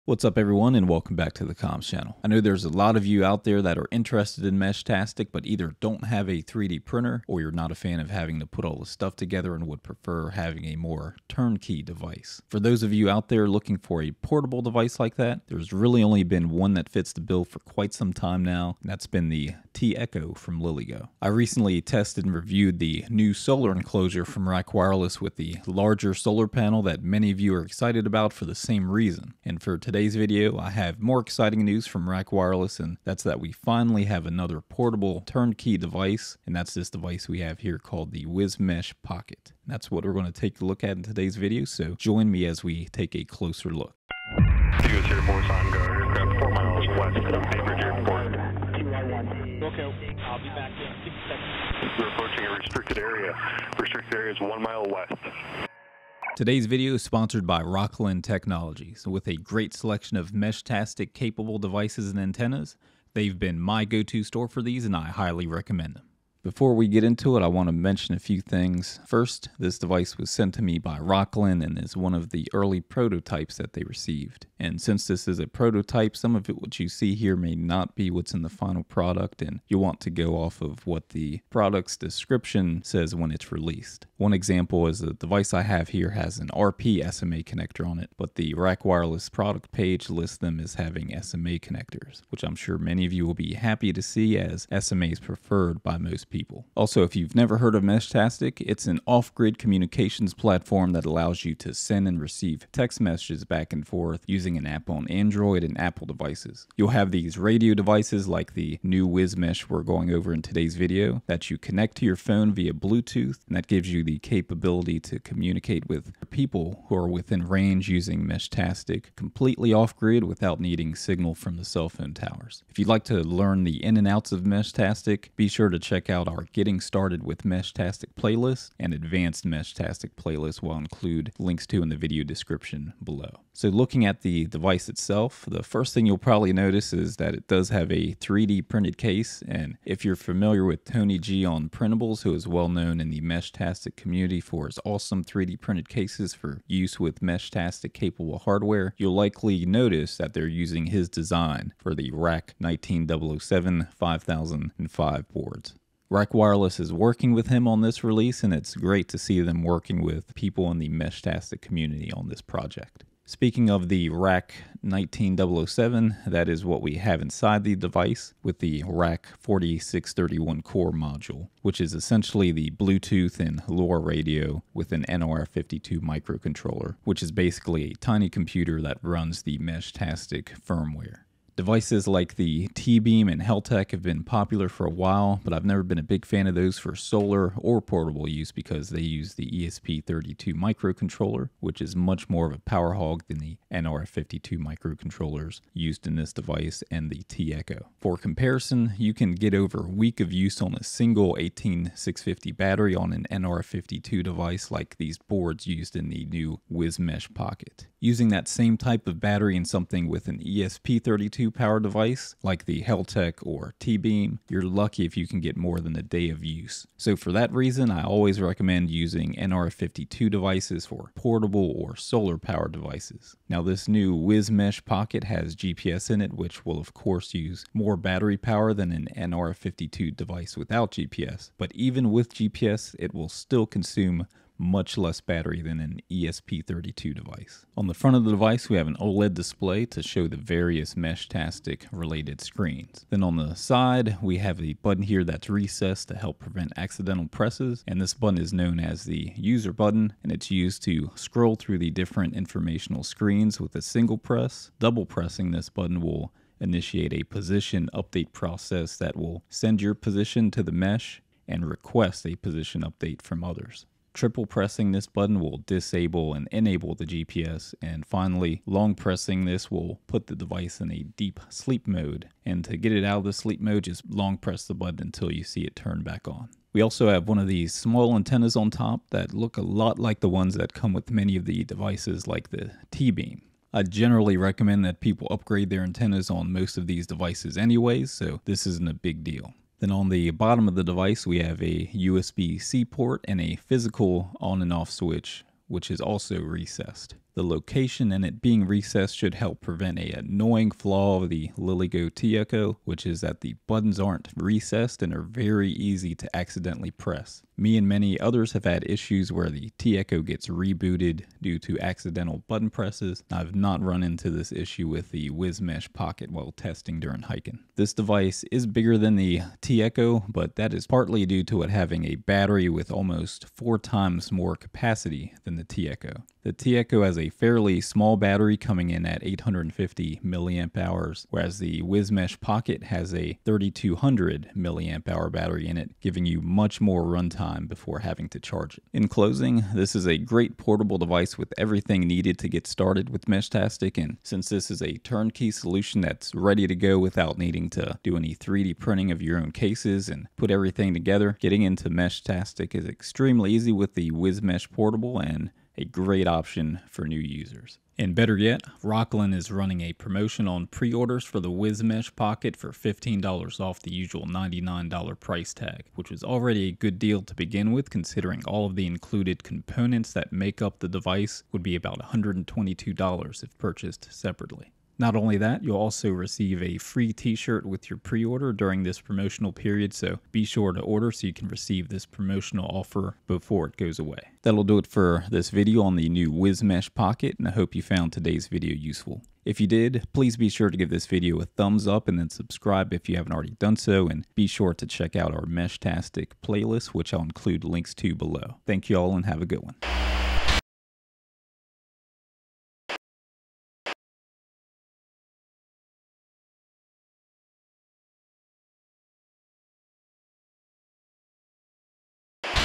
WisMeshPocket - The New Ready to go Meshtastic Portable from RAK Wireless!
Today's video is covering the new turn-key portable Meshtastic device from RAK Wireless called the WisMesh Pocket. It's currently available at Rokland with promotional pricing, so be sure to pick one up the link before the promotion is over!

Product Reviews: Products featured in this video were provided by the manufacturer for review. My opinions are my own and are not influenced by any sponsorship or partnership agreements.